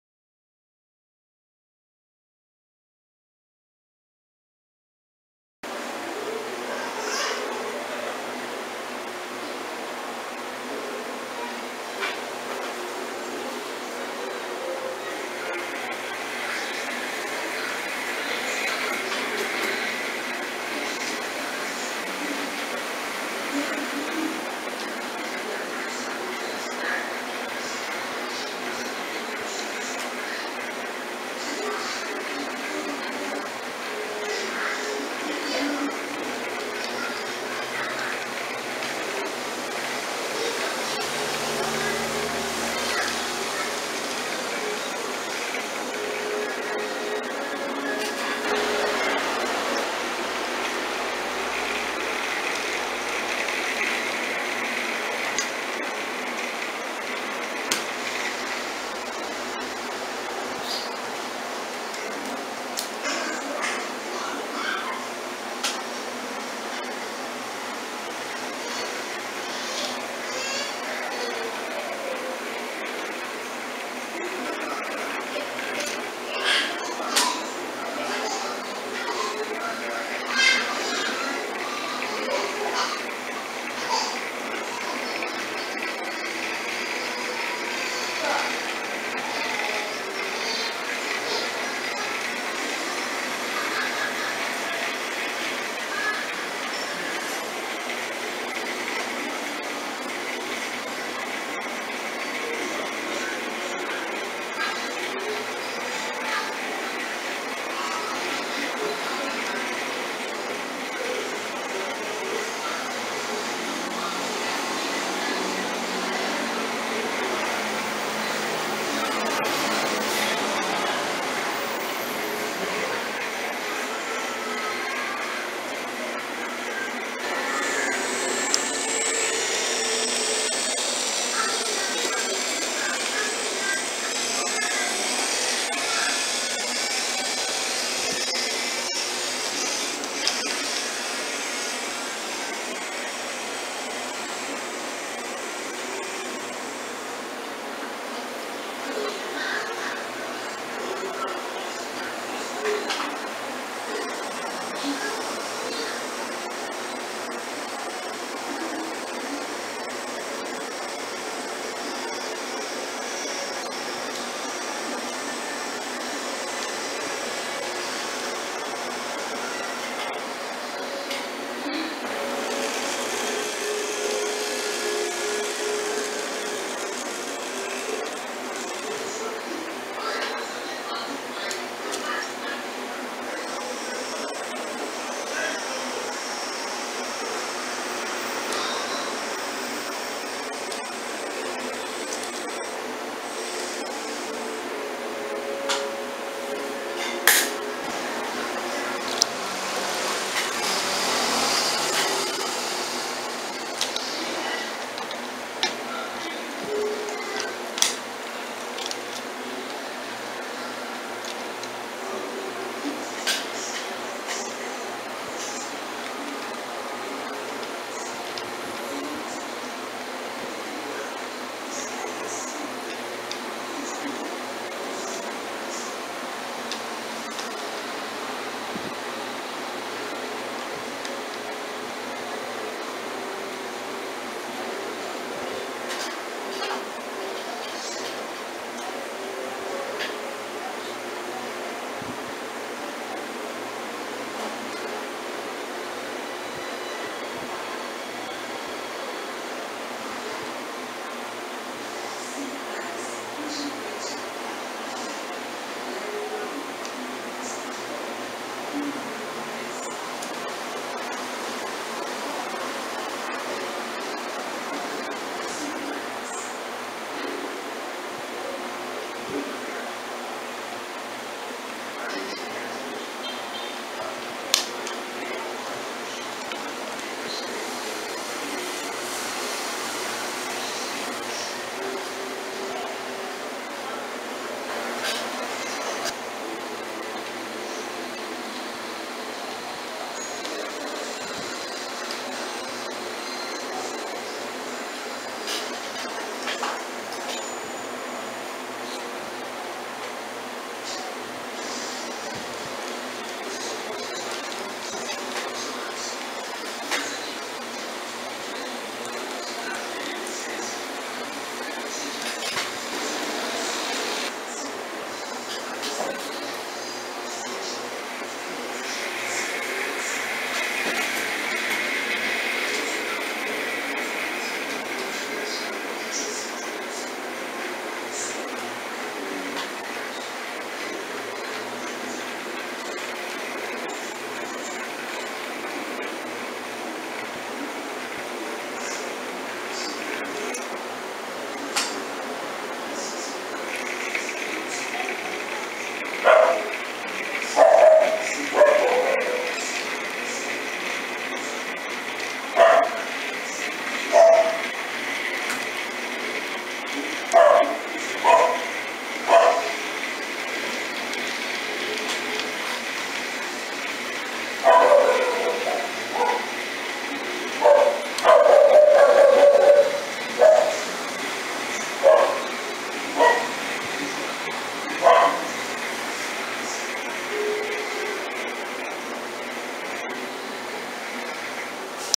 Manobra na maquete!
Pátio pequeno... muita manobra! Manobrando vagões pranchas da Frateschi com locomotivas Lambretinha (GE 70Ton), geezinha 45Ton e trackmobile.